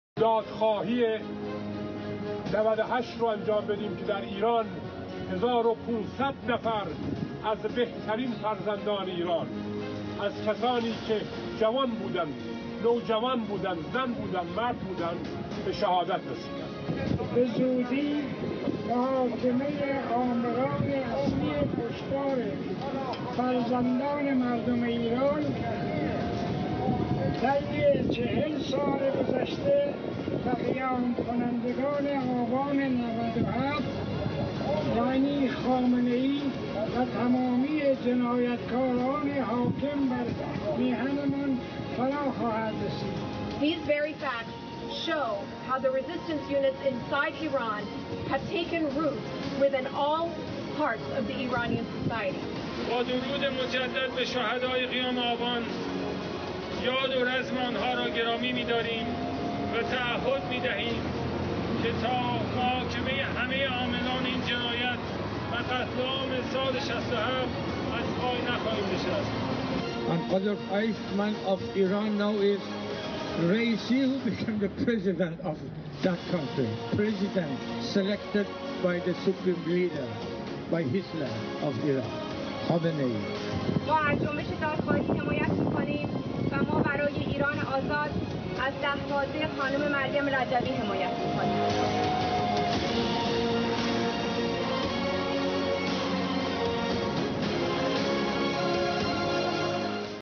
تظاهرات سراسری جنبش دادخواهی قتل‌عام ۶۷ - -گرامیداشت سالگرد قیام آبان۹۸
در سالگرد قیام آبا۹۸، ایرانیان آزاده، هواداران مجاهدین خلق ایران در ادامه کارزار جهانی، و فراخوان برای محاکمه خامنه‌ای و رئیسی جلاد بخاطر قتل‌عام سی هزار زندانیان سیاسی مجاهد، تظاهرات و تجمع در کشورهای مختلف برگزار کردند.